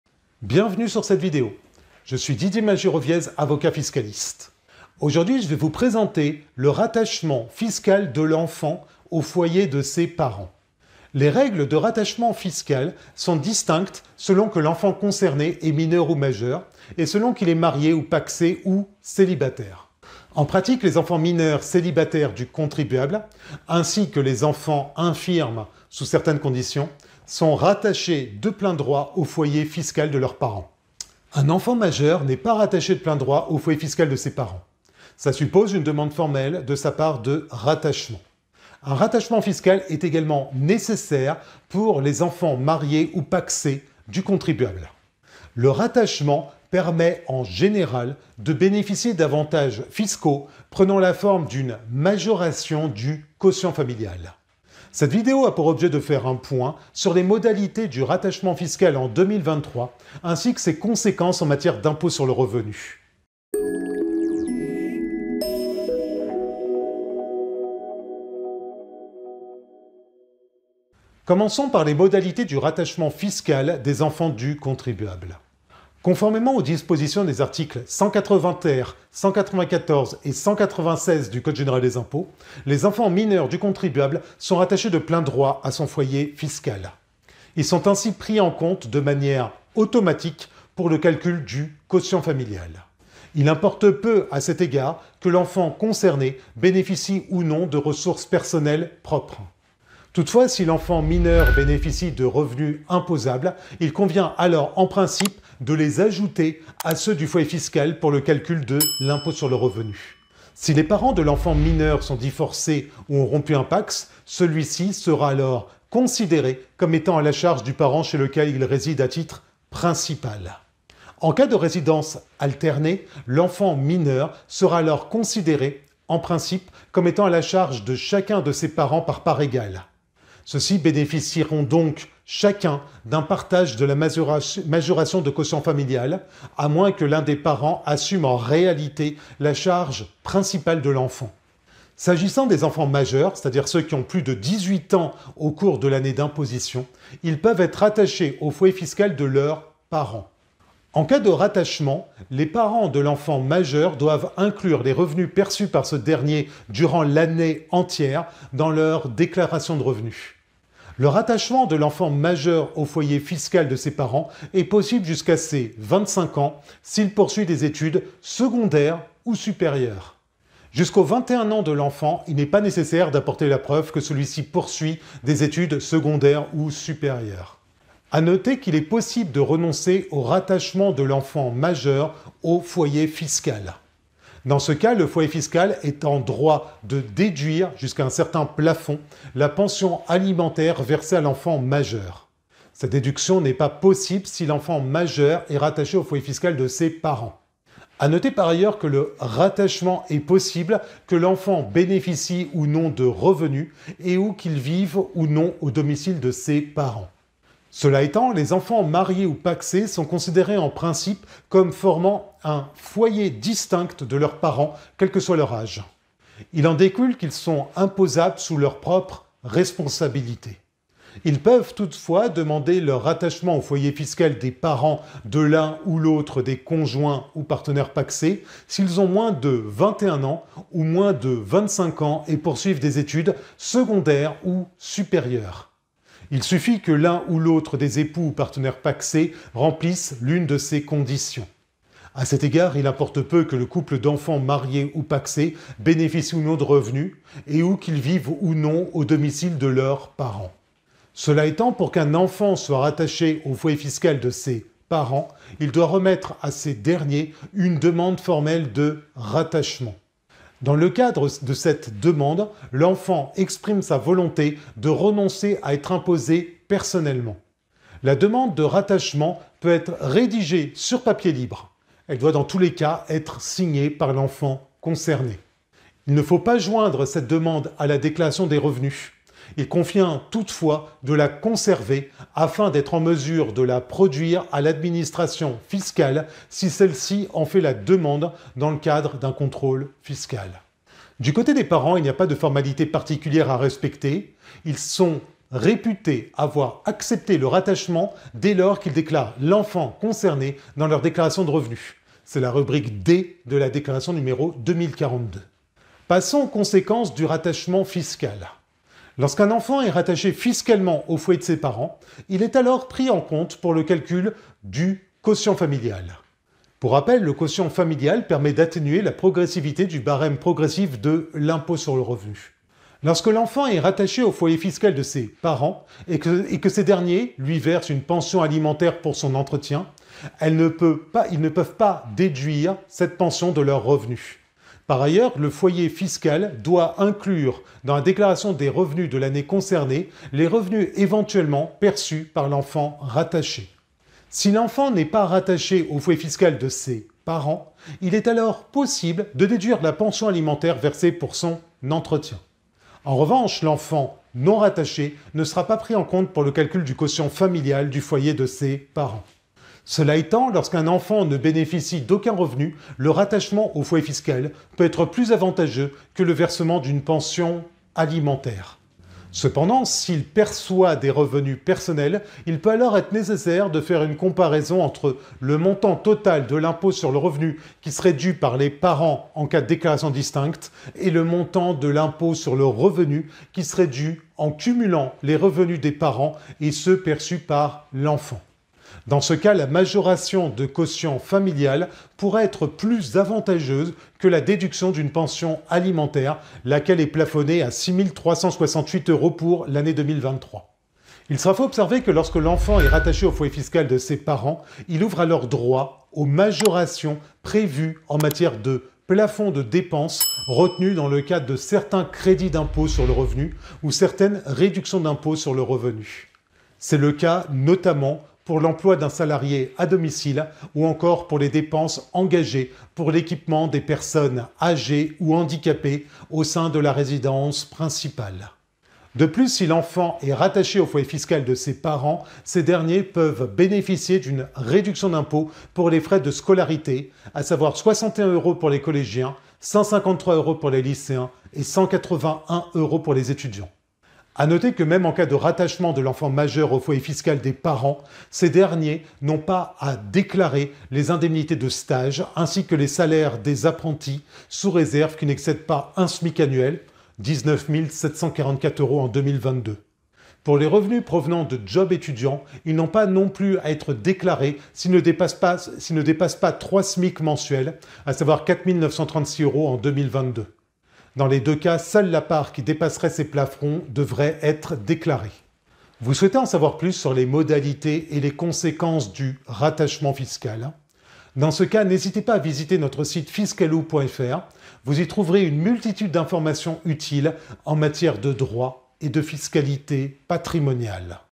Rattachement fiscal au foyer : conditions et conséquences
Le rattachement fiscal consiste à inclure dans le foyer fiscal un enfant.

Les règles sont distinctes selon que l’enfant concerné est majeur ou mineur, et, le cas échéant, selon qu’il est célibataire, marié ou pacsé.

En principe, un enfant majeur n’est pas rattaché de plein droit au foyer fiscal de ses parents. Il doit faire une demande formelle de rattachement.

Le rattachement offre la possibilité de bénéficier d’avantages fiscaux intéressants, prenant la forme d’une majoration du quotient familial.

Cette vidéo fait un point sur les modalités du rattachement fiscal en France en 2023, ainsi que ses conséquences en matière d’impôt sur le revenu.

00:00 Introduction
01:10 Modalités du rattachement fiscal des enfants du contribuable
05:05 Conséquences du rattachement fiscal
07:55 Vous souhaitez en savoir plus sur le rattachement fiscal ?

▬▬▬▬▬▬ INFORMATIONS DE CONTACT ▬▬▬▬▬▬▬▬

Maître Didier MAJEROWIEZ, Avocat Fiscaliste

👉 Pour en savoir plus sur ce sujet, nous vous invitons à consulter cet article

▬▬▬▬▬▬▬▬▬▬▬ VIDÉOS ASSOCIÉES ▬▬▬▬▬▬▬▬▬▬▬